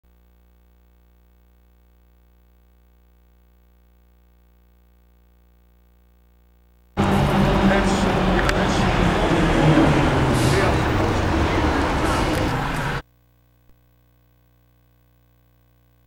RDMVTL 7599 ~ 3/31/2005 DRESSER RAND SMOKE STACK @ PAINTED POST , NY
PAINTED POST , NEW YORK ~ A clip of what remains of the smoke stack of the former Ingersol~Rand factory at Hamilton Street Route 417. Standing in the parking lot of Pierri's Central Restaurant  & The Sit N' Bull .